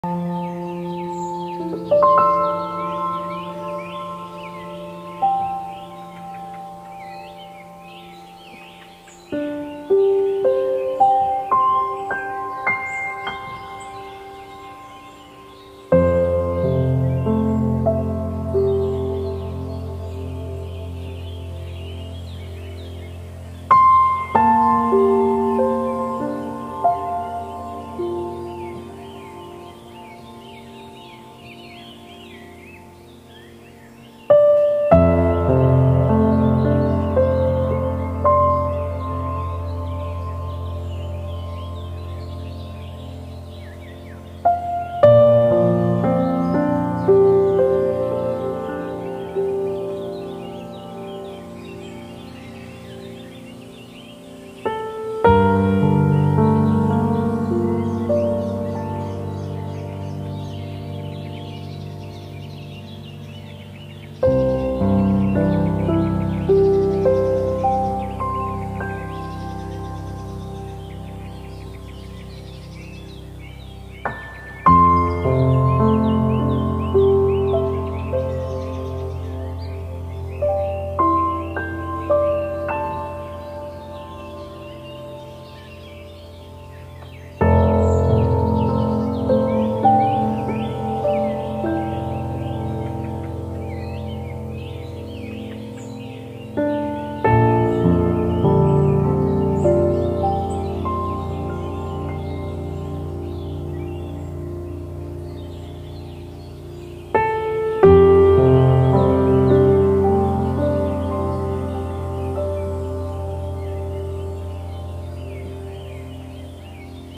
Tablerose blooming time lapse video/ portulaca blooming time lapse
in this video I have shown portulaca flower blooming video